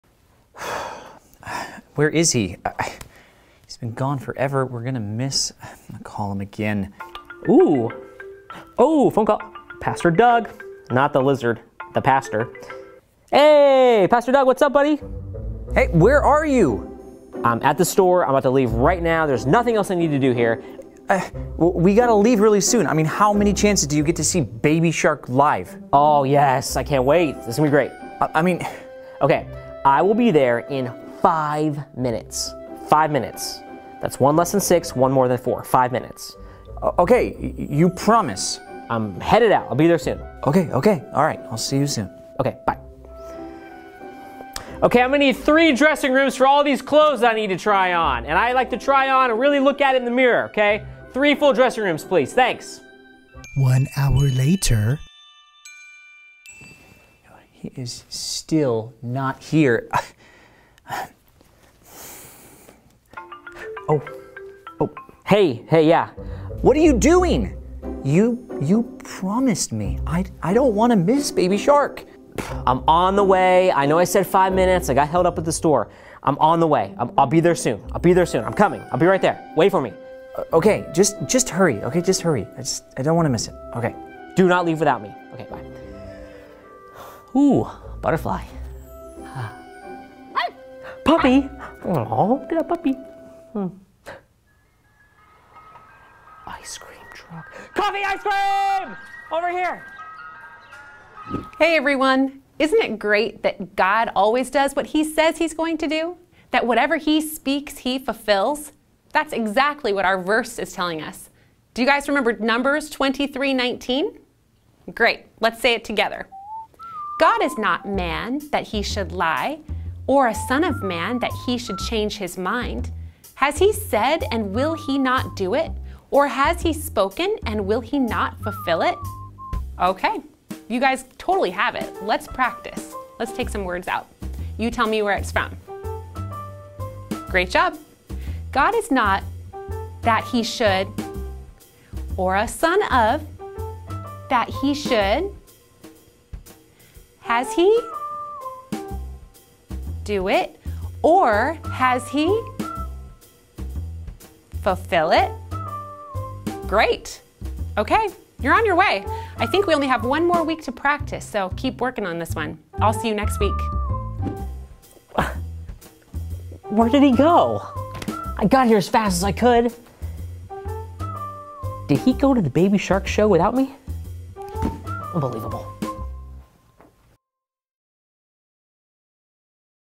Numbers 23:19 (Part 2) | Kids Bible Memory Verse | Compass Bible Church
Pastor Doug is waiting patiently for Brandon to arrive after promising he'd be there soon. But it becomes pretty clear that human promises, as we learn in Numbers 23:19, are nothing compared to God's promises! ‎